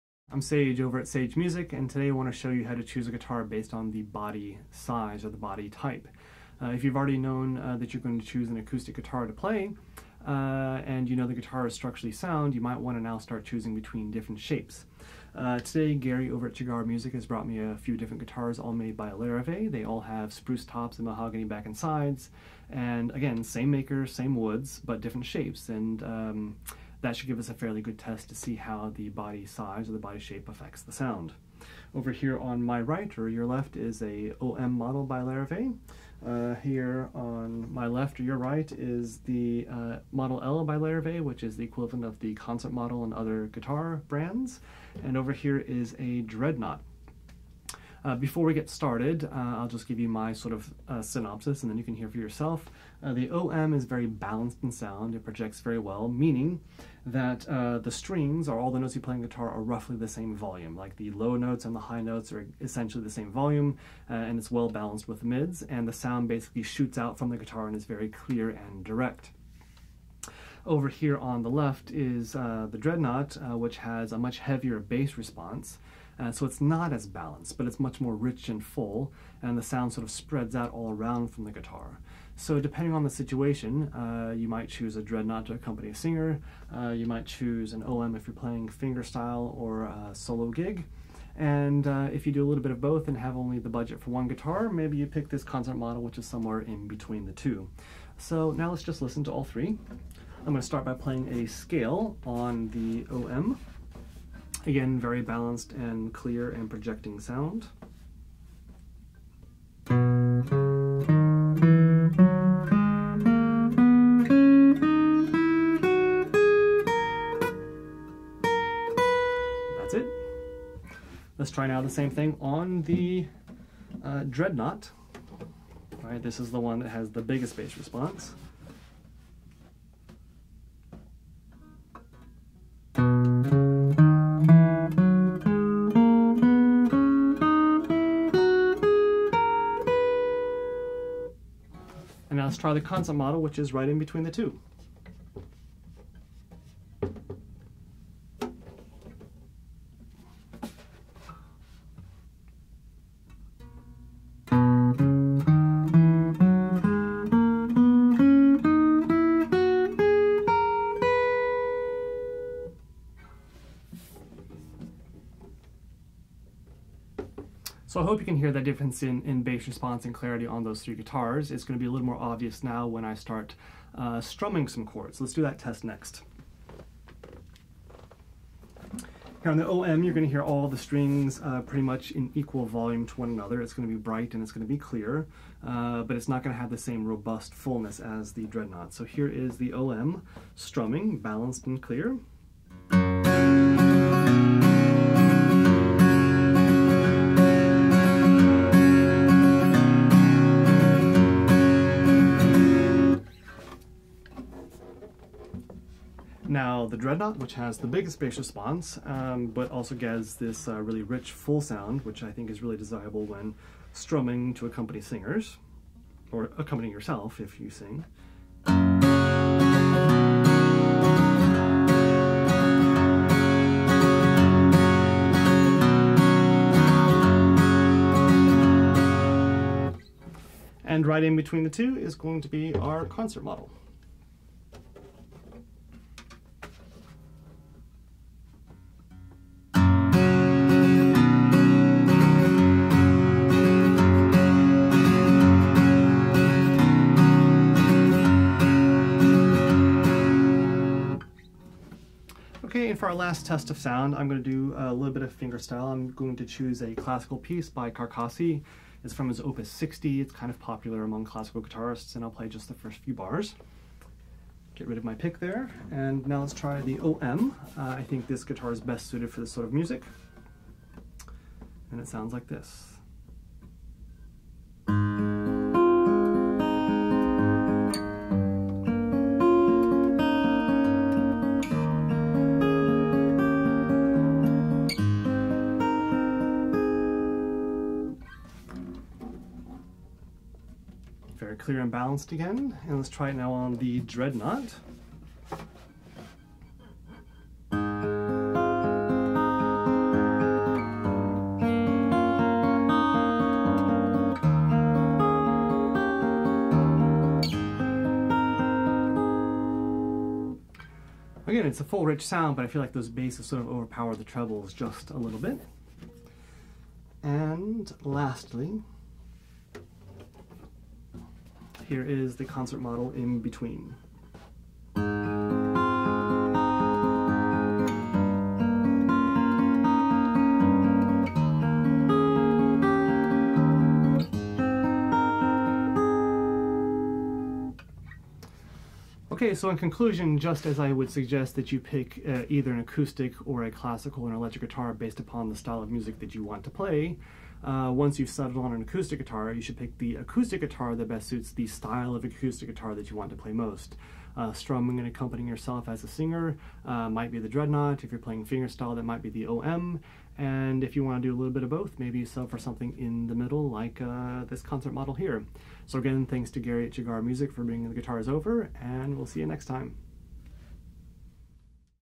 Choosing an Acoustic Guitar - Which Body Type to Choose?
The shape of the guitar's body, can have a big impact on the sound quality and your comfort level with the instrument. See how the body shape may affect the sound of the guitar. For a more detailed analysis, see our post